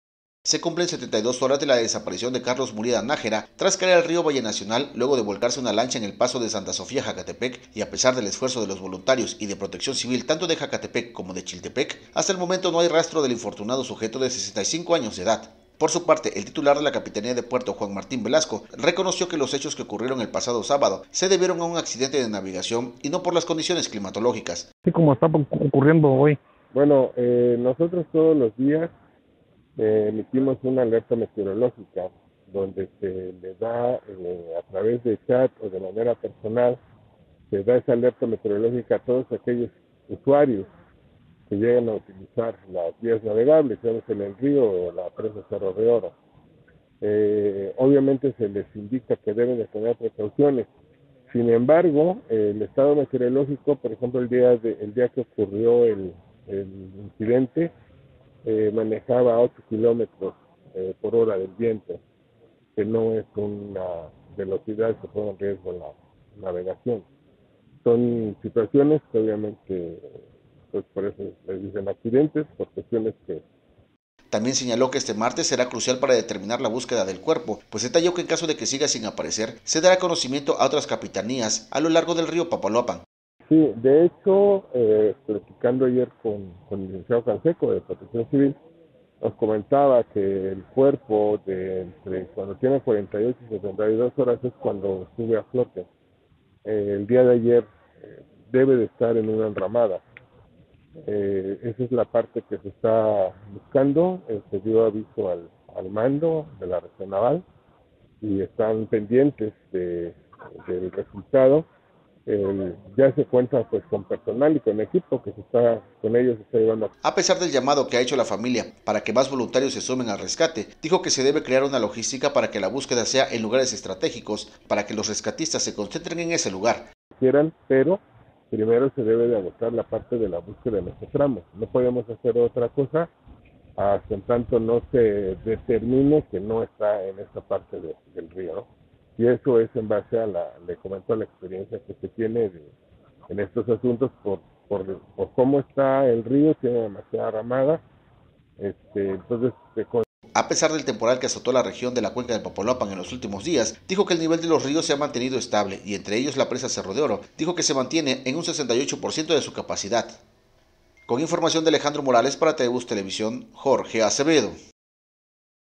Hundimiento de lancha en rio Valle Nacional, fue accidente de navegación: Capitanía de Puerto
🔹 Se cumplen 72 horas de la desaparición de Carlos Murieda Nájera, tras caer al río Valle Nacional al volcarse una lancha en el paso de Santa Sofía Jacatepec y a pesar del esfuerzo de voluntarios y protección civil de Jacatepec y Chiltepec, hasta el momento no hay rastro del infortunado sujeto de 65 años de edad.